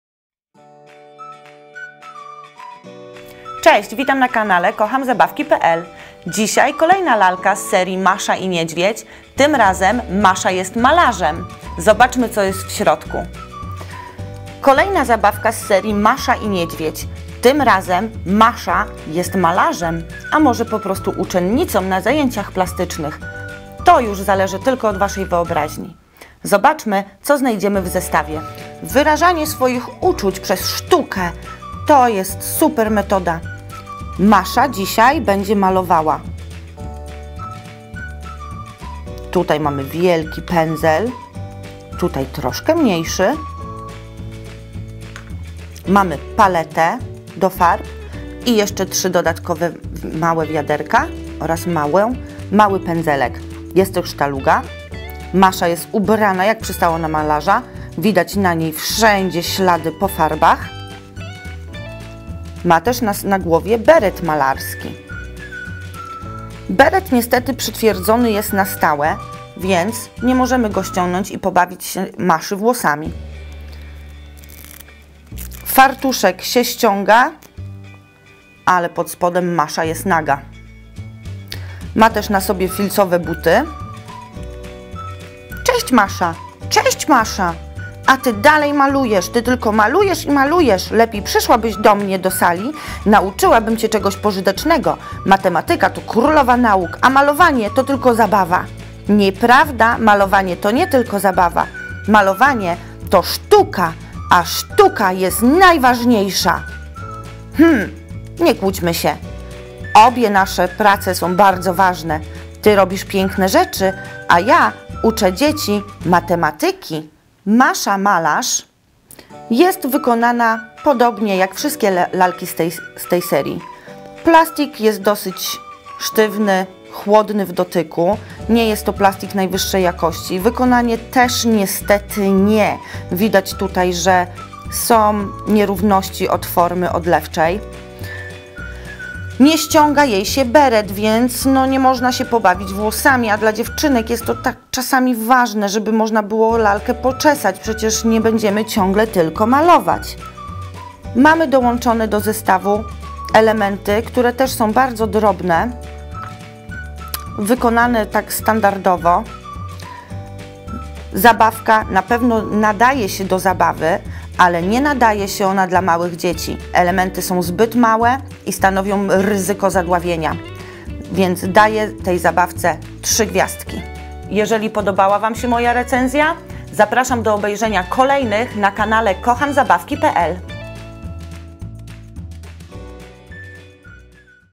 Masha and the Bear / Masza i Niedźwiedź - Simba - Masha Paint Fun / Masha jako Artysta Malarz - 2047
Masza i Niedźwiedź - Masza jako artysta malarz - zestaw z mini laleczką i akcesoriami malarskimi od firmy Simba.

Paint with Masha! This playset in inspired by Masha and the Bear's TV show, from the episode in which Masha discovers her skills as a painter. The playset includes Masha's doll dressed with a pink coat, 4,72" tall, and many painter accessories like colors, brushes and a canvas. Recommended age: 3+.